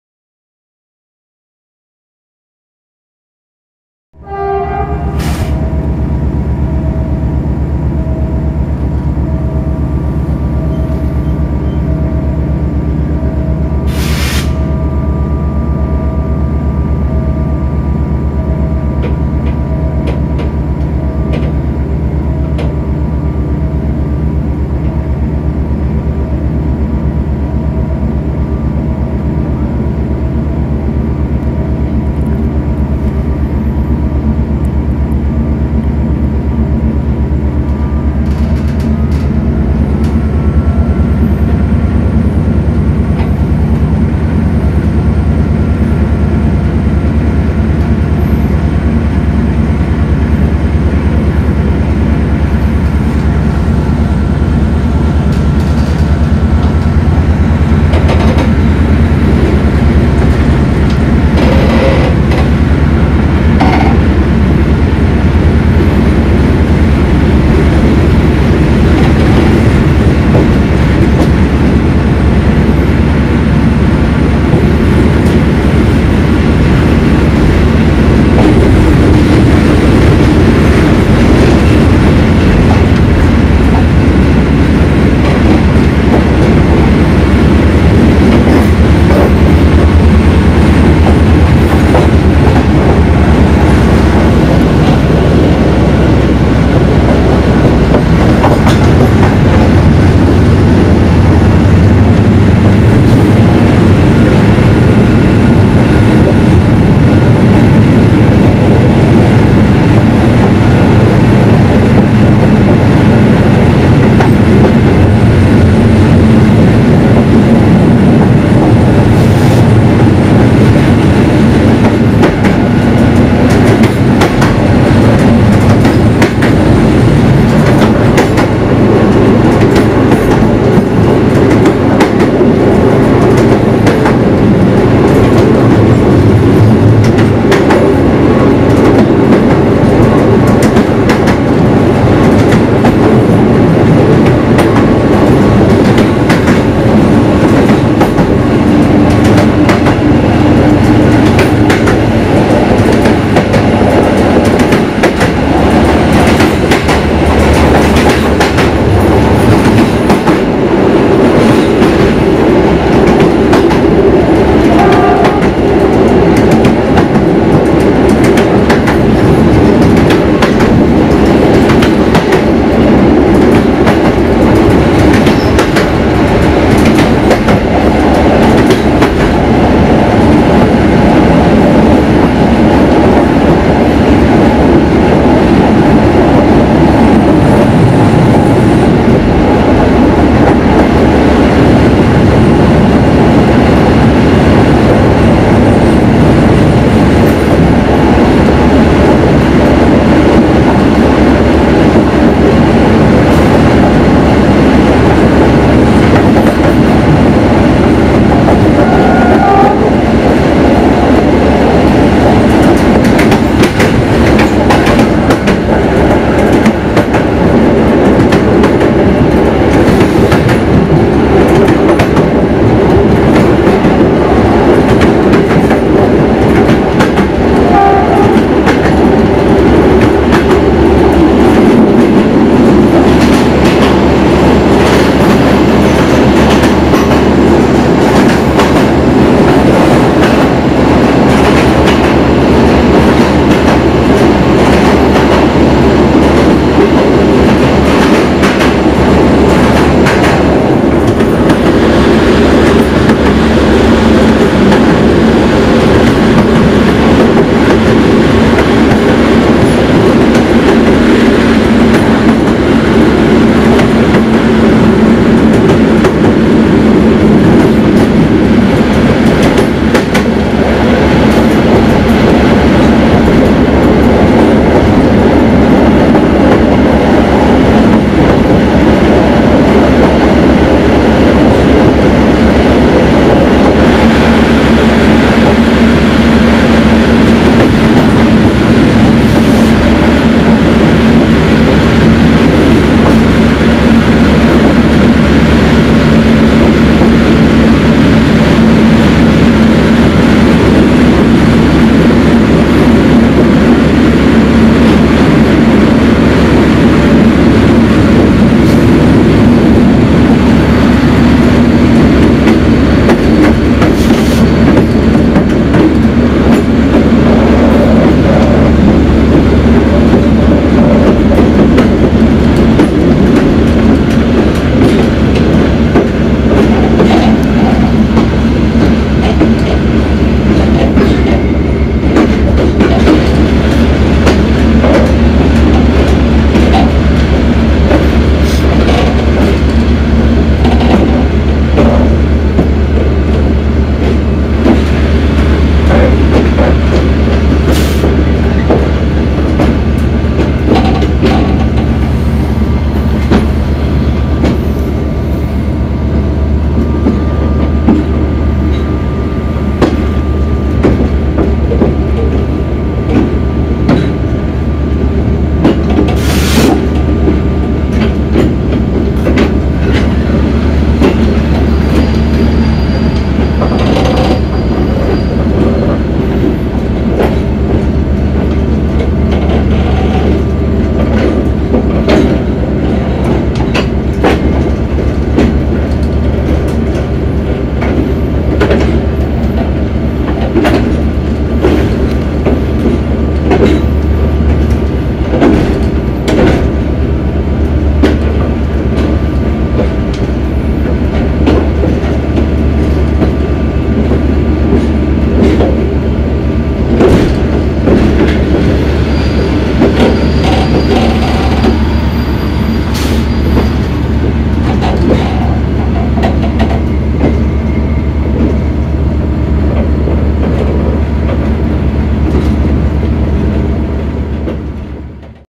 Calatorie cu P3645 pe ruta Balnaca - Suncuius (Winter Edition) (20 02 2011)
Trip with Regional Train no. 3645 on route Balnaca - Suncuius (Winter Edition) (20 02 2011)

1: P3645 - Bratca - Oradea - Porneste din Balnaca tras de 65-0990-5
2: P3645 - Bratca - Oradea - Intra in Suncuius

(Winter Edition)